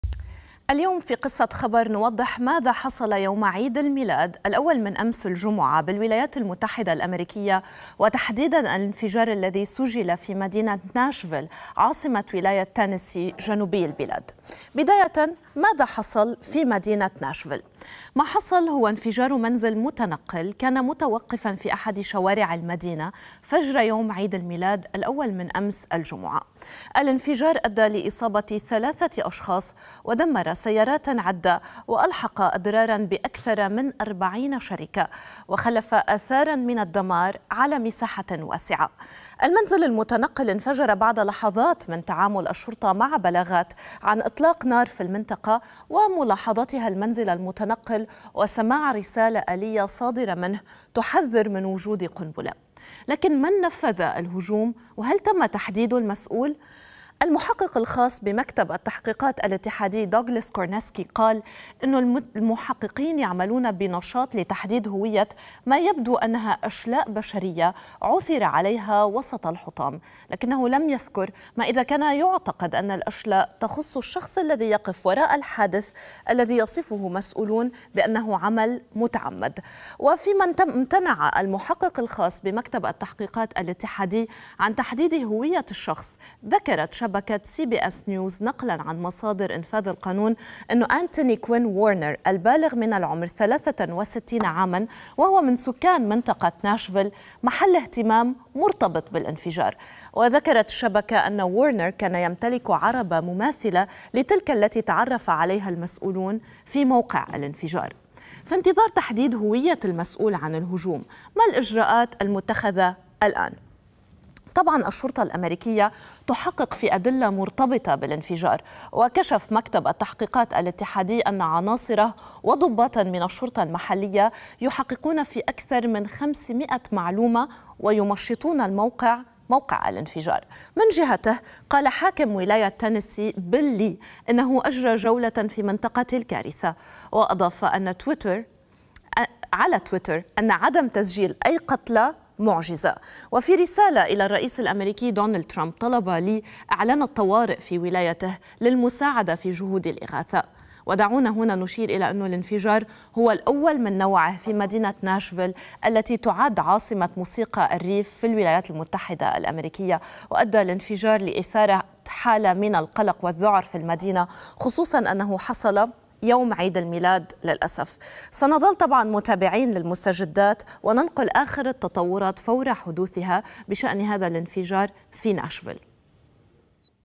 ماذا حصل في عاصمة موسيقى الريف في الولايات المتحدة؟
اليوم في "قصة خبر"، نوضح ماذا حصل يوم عيد الميلاد، الأول من أمس الجمعة، في الولايات المتحدة، وتحديدا الانفجار الذي سُجل في مدينة ناشفيل، عاصمة ولاية تينيسي، جنوبي البلاد. بدايةً، ماذا حصل في مدينة ناشفيل؟ ما حصل هو انفجار منزل متنقل، كان متوقفا في أحد شوارع المدينة، فجر يوم عيد الميلاد، الأول من أمس الجمعة. الانفجار أدى لإصابة ثلاثة أشخاص، ودمّر سيارات عدة، وألحق أضرارا بأكثر من أربعين شركة، وخلّف آثارا من الدمار على مساحة واسعة. المنزل المتنقل انفجر بعد لحظات من تعامل الشرطة مع بلاغات عن إطلاق نار في المنطقة، وملاحظتها المنزلَ المتنقل، وسماع رسالة آلية صادرة منه، تحذّر من وجود قنبلة.